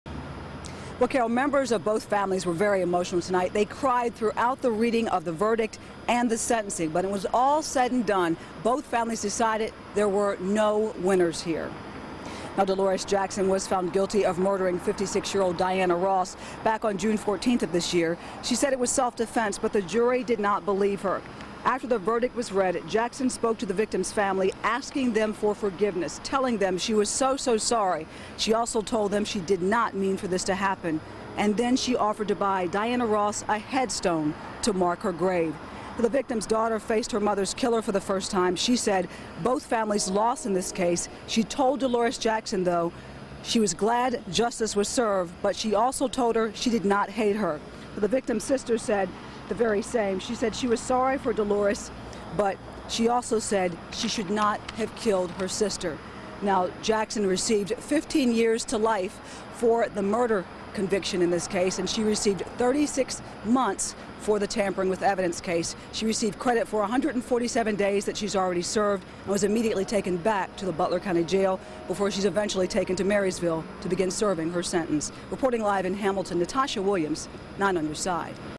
72-year-old Delores Jackson found guilty
Jackson faces 15 years to life in prison